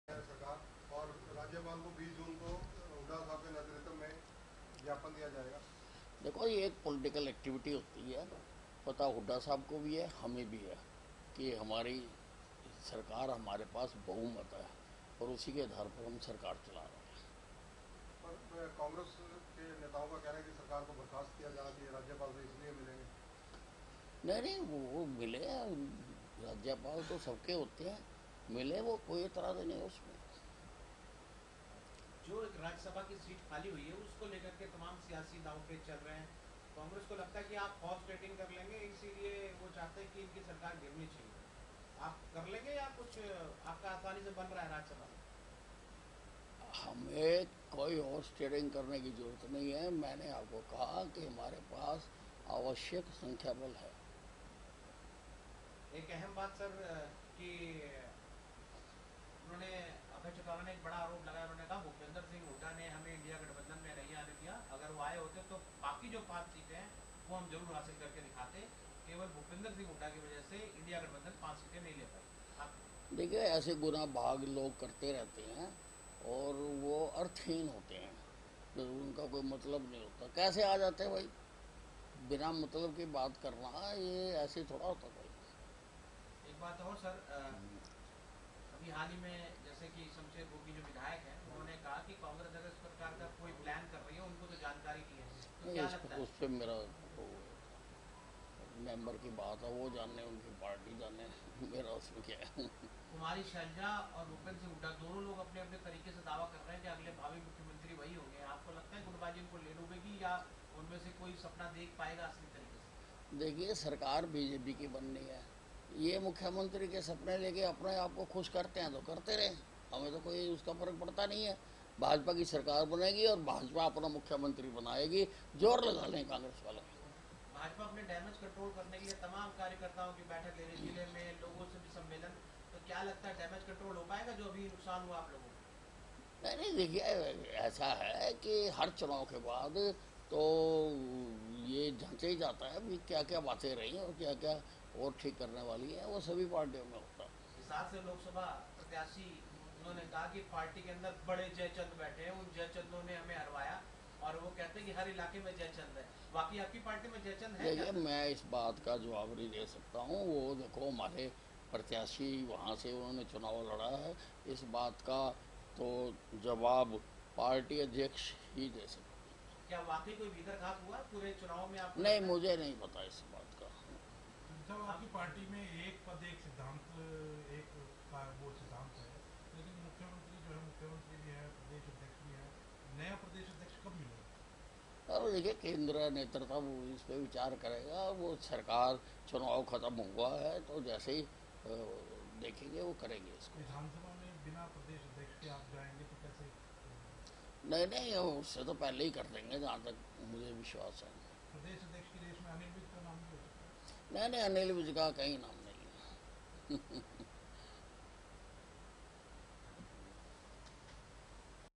कांग्रेस की गुटबाजी को लेकर विज का हुड्डा पर कटाक्ष
कांग्रेस की गुटबाजी को लेकर विज का हुड्डा पर कटाक्ष,बोले तीसरी बार फिर बनेगी हरियाणा में भाजपा की सरकार